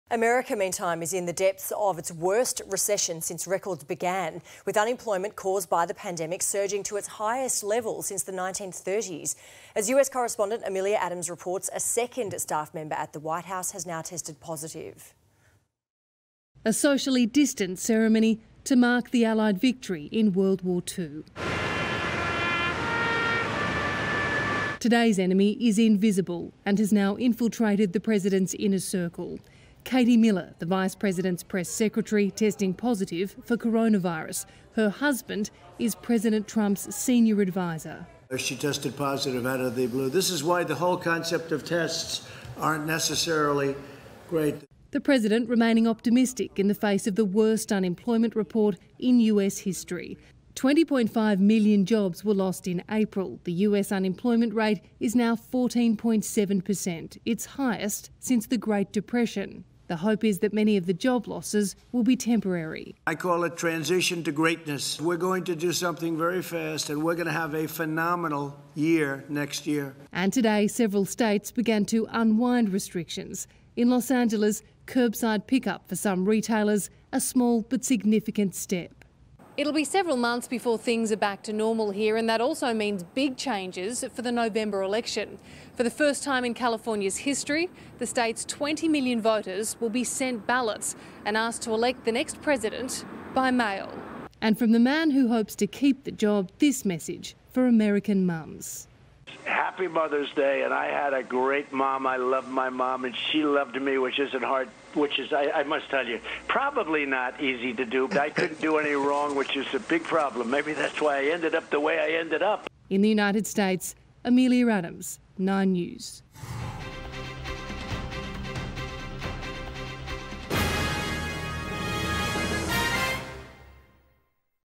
Coronavirus: America sinks into its worst recession | Nine News Australia
President Donald Trump has promised a “transition to Greatness” even as America’s unemployment rates continue surging to levels not seen since the Great Depression of the 1930's.